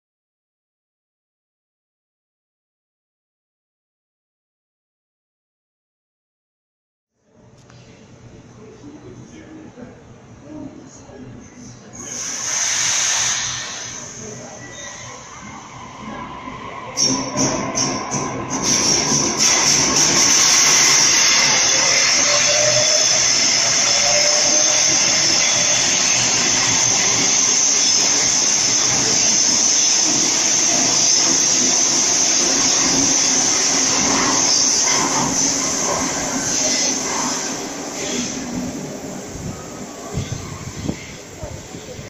(1900th video!) Sydney Trains B1 at Martin Place P1
As the S sets will soon be phased out, the B sets are undergoing tests within the Sydney Trains network since April this year
This was filmed during my time in Sydney on 15/6/18